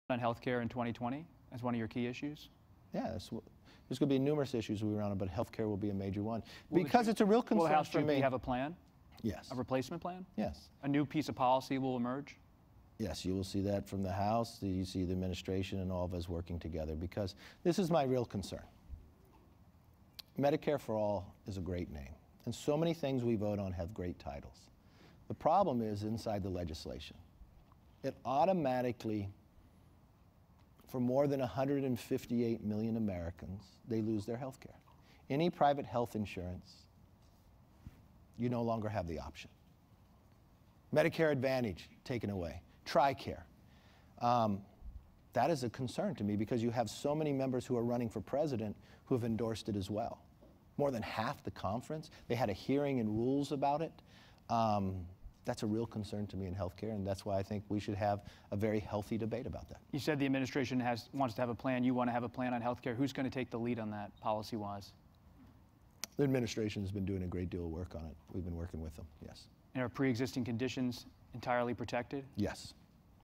House Minority Leader Kevin McCarthy says Administration will have ACA replacement
Republican Minority Leader Rep. Kevin McCarthy said Republicans and the Trump Administration are working on a replacement plan for the Affordable Care Act. “Yes, you will see that from the House and the administration, all of us working together.”